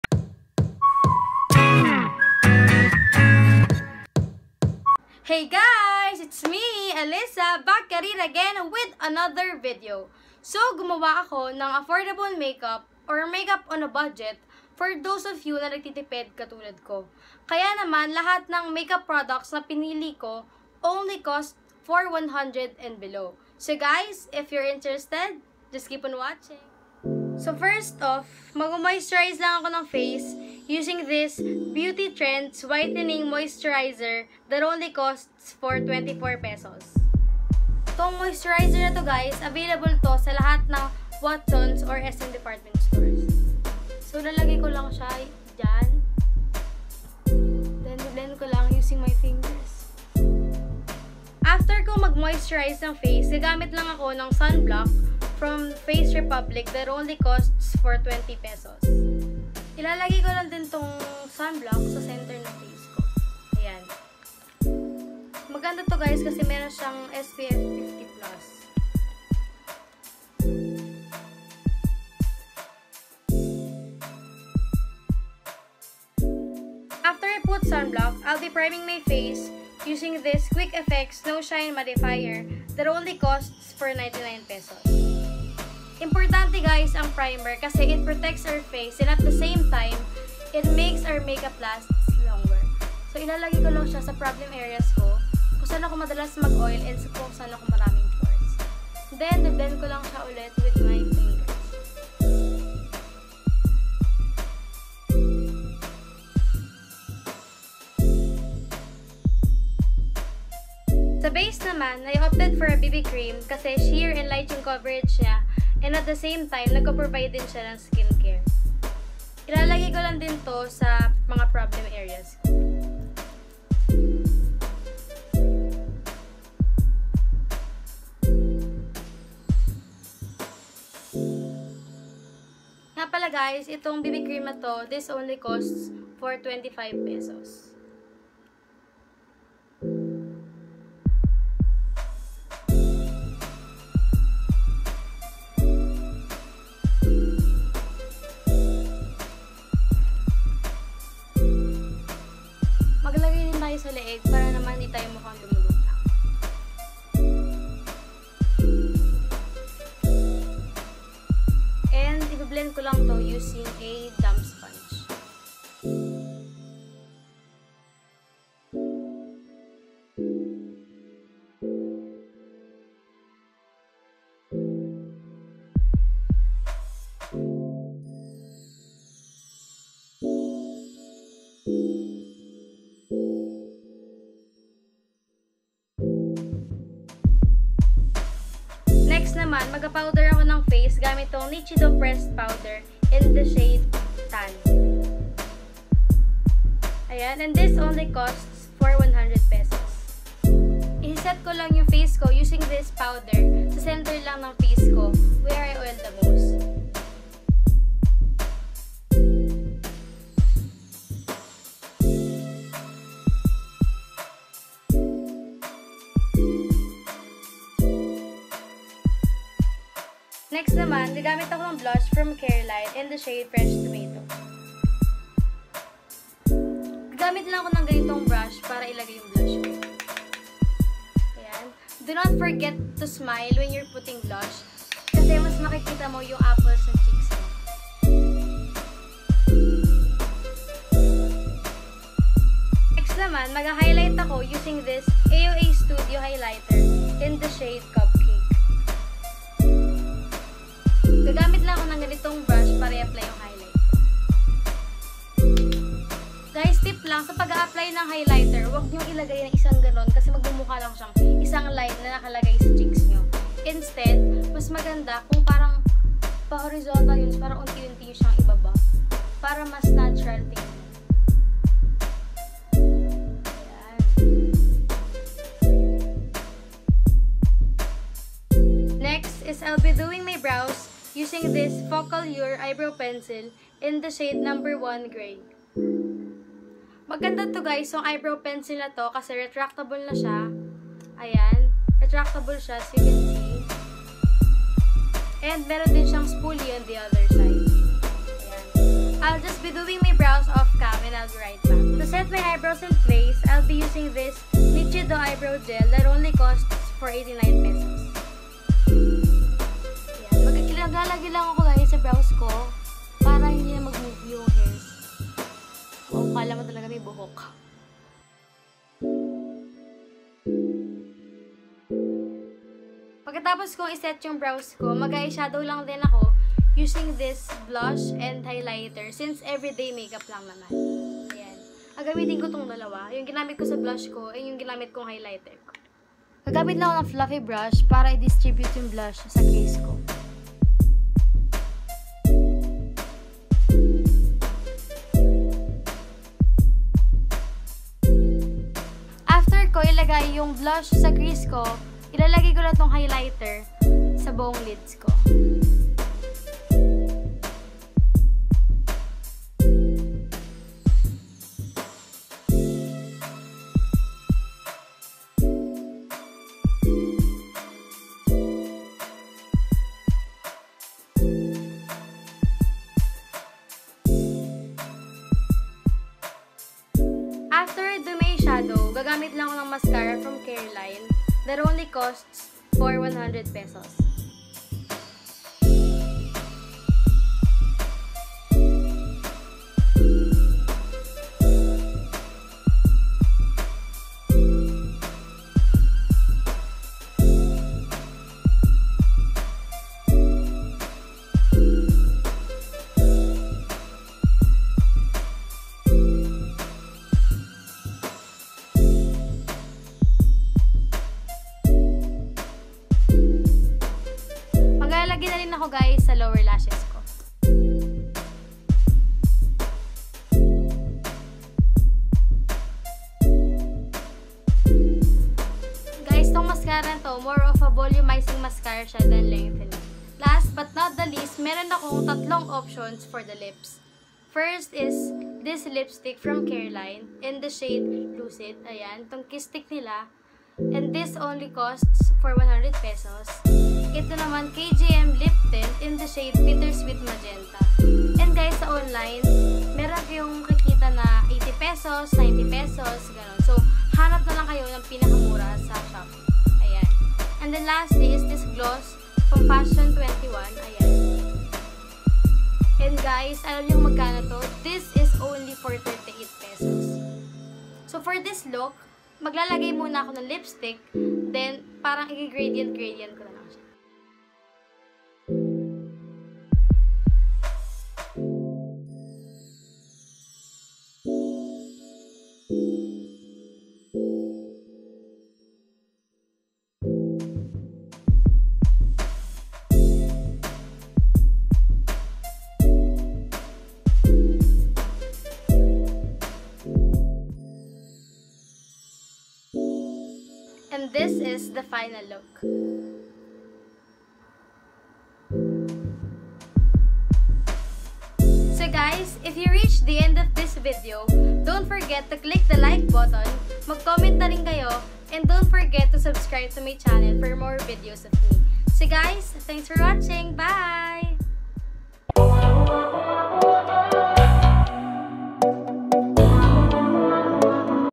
Makeup on a Budget?!
*I DO NOT IN ANY WAY OWN THE MUSIC ON THIS VIDEO*

PRODUCTS MENTIONED:
Beauty Trends whitening moisturizer- 24php
Face republic sunscreen- 20php
Quick fx no shine mattifier- 99php
Iwhite bb.holic in light- 25php
Nichido pressed powder in tan - 100php
Careline blush in fresh tomato- 95php
Aoa highlighter in cupcake- 98php
Focallure eyebrow pencil in no.1 (gray) -71php
Nichido eyebrow gel- 89php
Careline go big mascara- 100php
Careline kiss stick in lucid- 100php
Kjm liptint in bittersweet magenta- 80 to 90php
Fashion 21 lipgloss - 38php